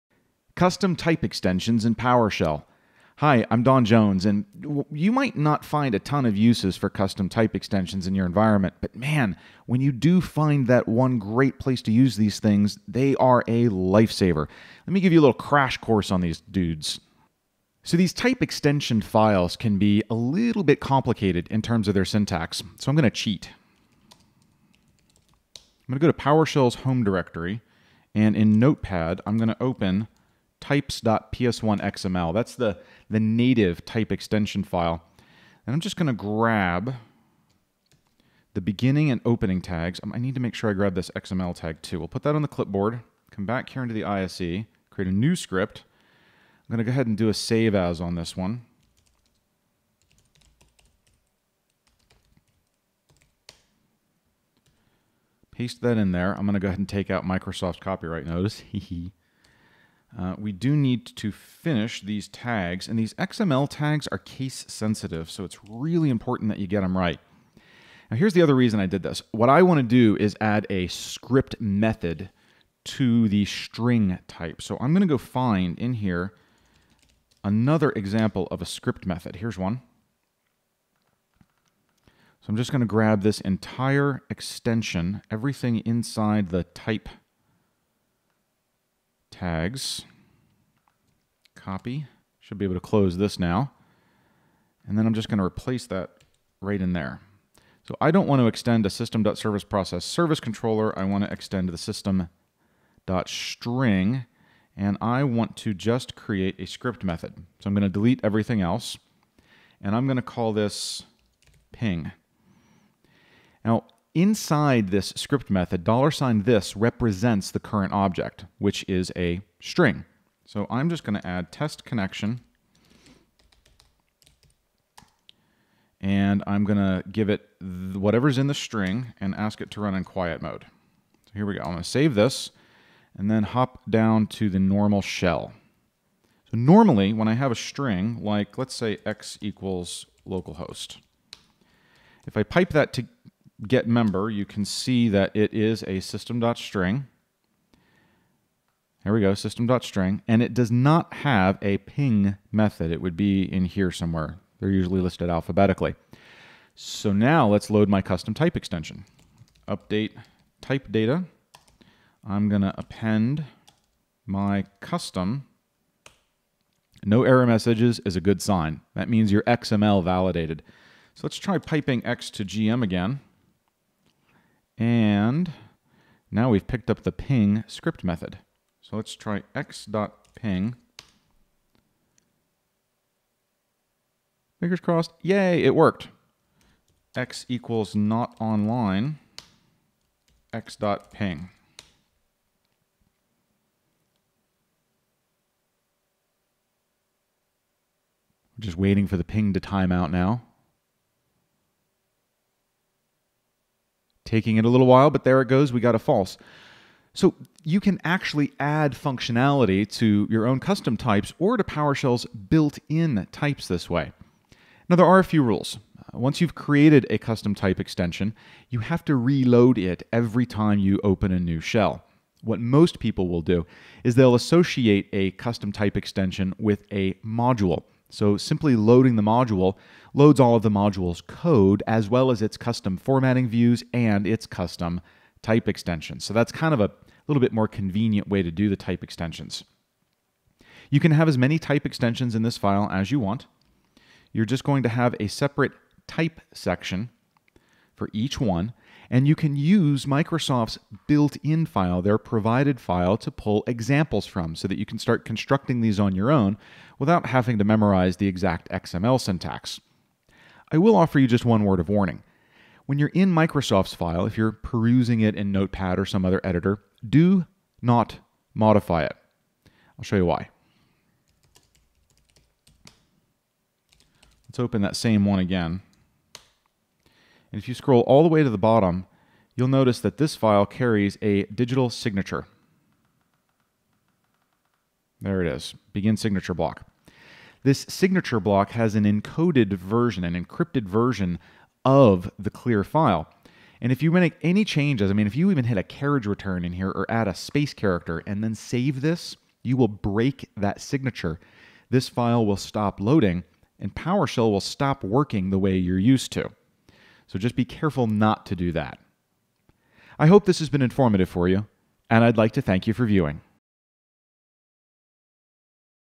MicroNugget: How to Create Custom Type Extensions in PowerShell
In this video, Don Jones covers creating custom type extensions in PowerShell. At first, you might not think that there are a lot of uses for custom type extensions in your environment. But the funny thing about them is when they're useful, they're very useful.

Many of PowerShell's cmdlets are powerful and helpful. Add-Member, for instance, can help you to add custom members to individual objects. But you still need to add each member that you want to interact with to each object you're using. You can't natively add them automatically to all objects of that type.

But that's why there are custom type extensions. These simple XML files allow you to add your own features to any type. That means that if you write code that interacts with a single type of object, you might be better off writing it as an extension to the type instead.

Watch along as Don finds templates for writing your type extension files — which makes it easier to keep the complicated syntax straight.

Watch how to write a simple extension and see how much versatility they can bring to your environments.